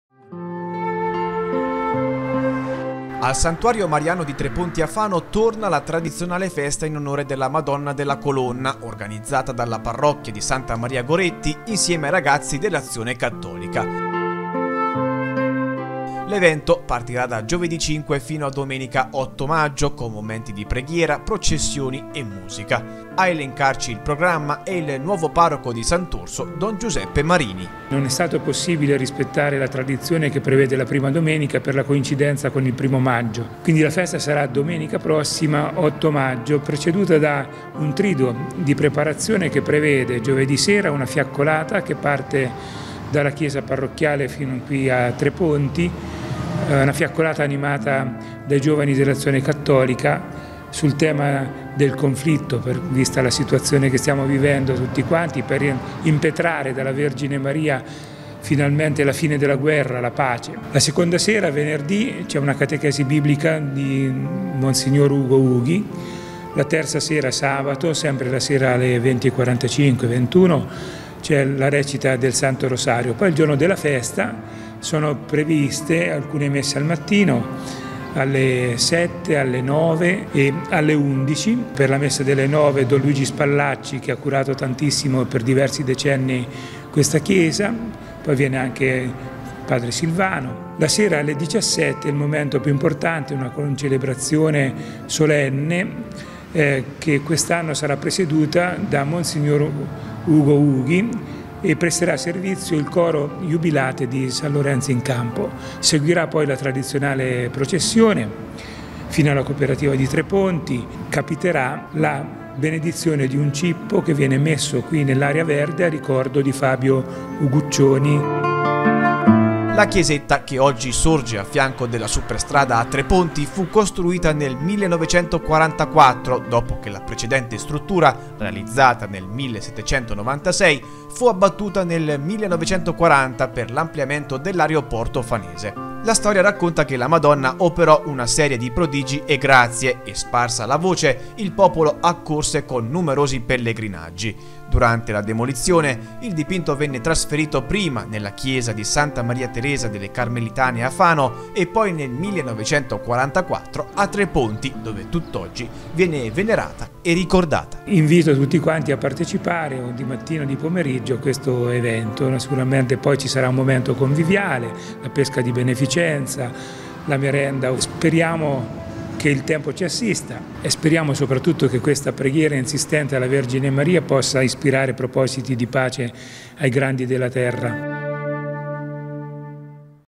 Fano, l'8 maggio la Festa della Madonna della Colonna
Prenderà il via con la fiaccolata per la pace la Festa della Madonna della Colonna ai Tre Ponti di Fano. Domenica le Sante Messe, la processione e la benedizione di un cippo in memoria di Fabio Uguccioni.